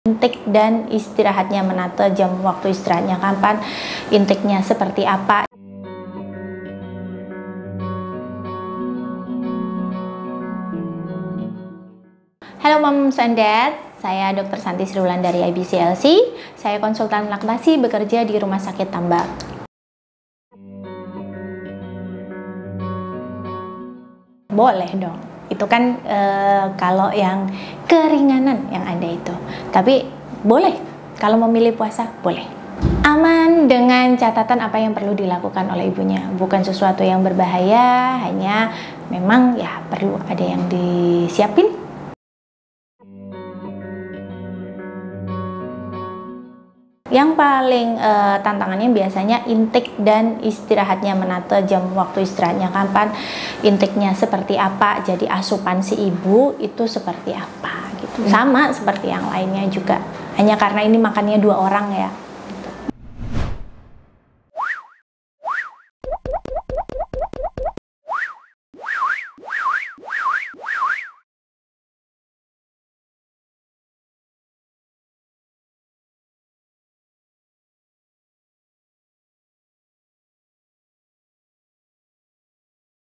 Ibu Menyusui Boleh Puasa? Ini Syaratnya Kata Dokter
Pada dasarnya, wanita yang sedang menyusui dibolehkan untuk tidak berpuasa jika tidak sanggup. Namun, jika hendak berpuasa pun, dokter konselor laktasi, dr. Santi Sri Wilandari, IBCLC menjelaskan syarat-syaratnya.

00:00 Intro
00:23 Bolehkah ibu menyusui puasa?
00:42 Apa saja yang harus diperhatikan?

ADVERTISING